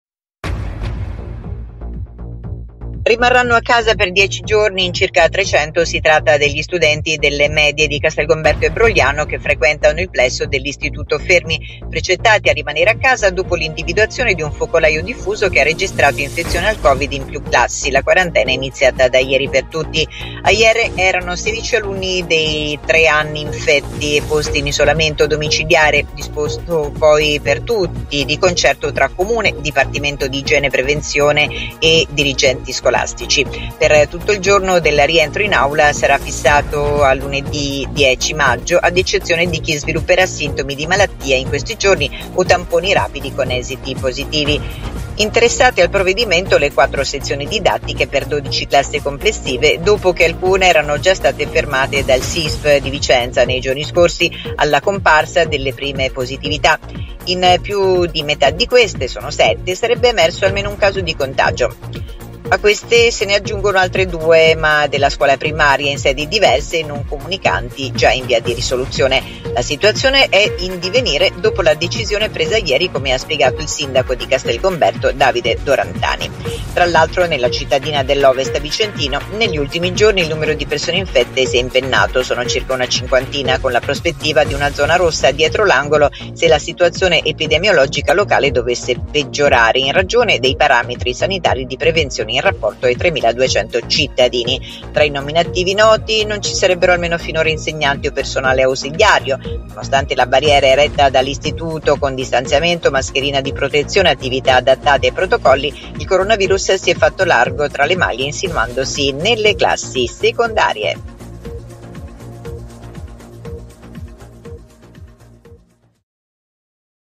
Scuole medie chiuse per Covid: 16 i contagi tra studenti di 7 classi. A casa in 300
Da ieri e fino a lunedì 10 maggio tutte e 12 le classi della scuola secondaria saranno chiuse e gli alunni in didattica a distanza. In sette di esse sono stati riscontrati dei casi di contagio, costringendo ad applicare il protocollo di prevenzione per le scuole. La decisione del Sisp di Vicenza è di martedì sera, in accordo con sindaco locale e direzione scolastica.